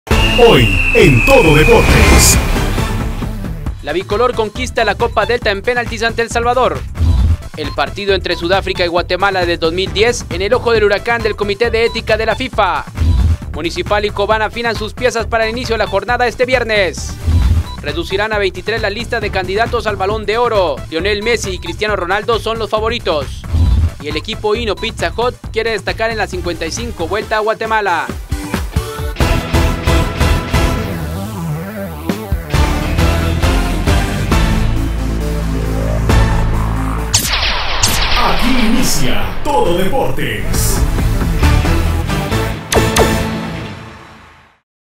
Titulares TD 14 de octubre
Guatemala cierra preparación previo a fase de grupos de eliminatoria mundialista con victoria en penaltis ante El Salvador | Suspenden a exdirectivo sudafricano relacionado con supuesto amaño del juego entre Sudáfrica y Guatemala | Municipal y Cobán se preparan para el partido del viernes | Reducirán la lista de candidatos al Balón de Oro.